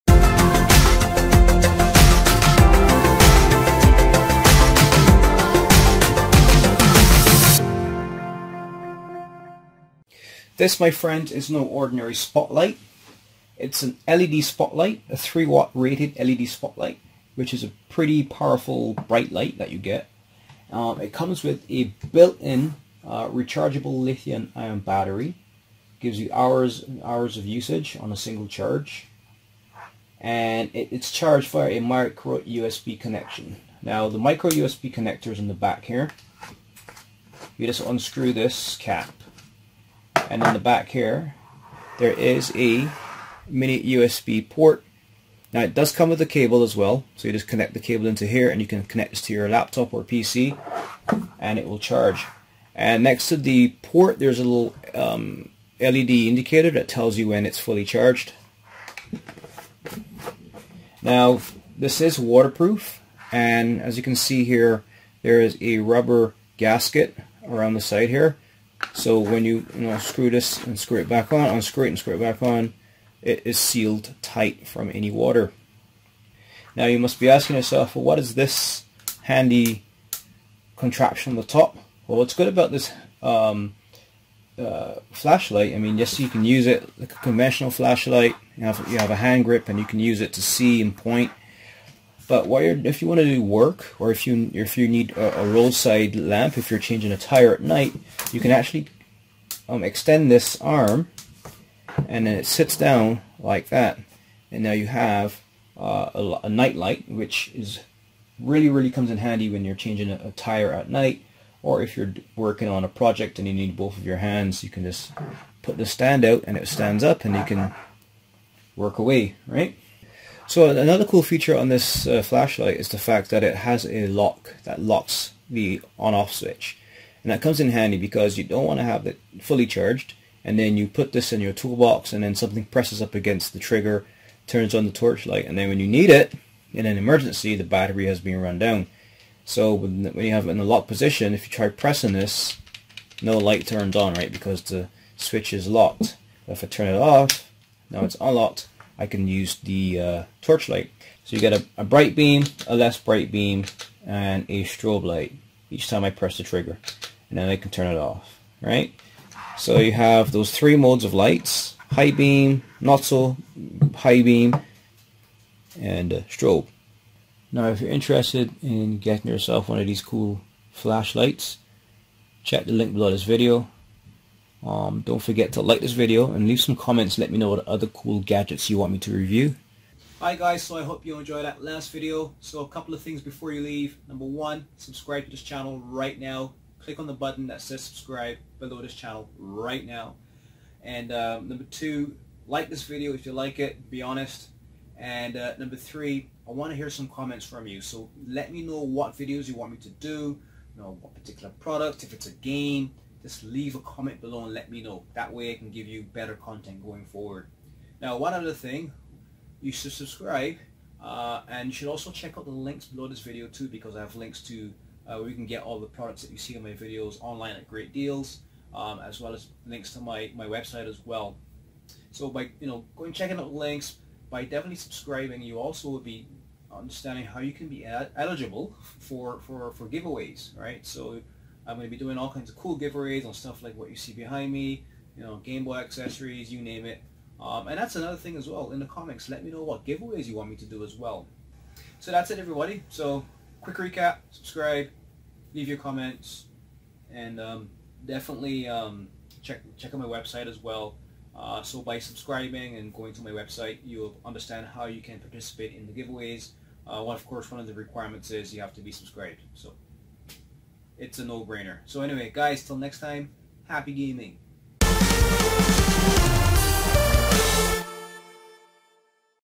Cheap, Super Bright, Ultra-Portable and Waterproof LED Flashlight Review Video (2019)
Get the LED ultra-portable LED flashlight here:

This portable LED flashlight is waterproof and also comes with a built-in lithium-ion battery that can be recharged via a mini USB connection located at the back of the device.  The LED flashlight can be recharged via your laptop's USB port or inside of your car as well if there is a USB port available for charging small electronics.

Another added feature is the adjustable kickstand which makes this ideal gadget to have in your car as it can provide light in situations when you have to repair a flat tire in the dark or look inside of your engine bay at night for example.